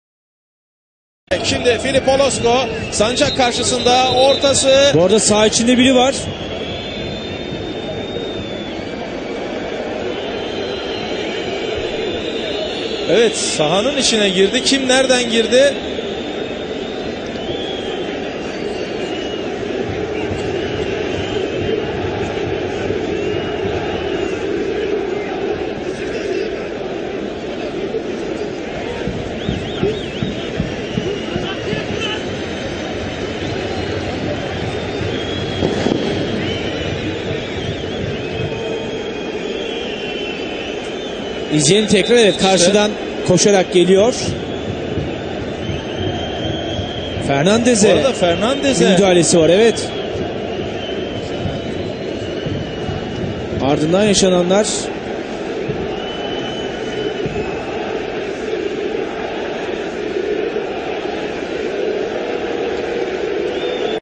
Hincha entra al partido y comete Penal
Un aficionado salta al campo de juego y derriba a un jugador en el area , lo que genera un caos el el partido entre Kasimpasa-Besiktas en Turquia.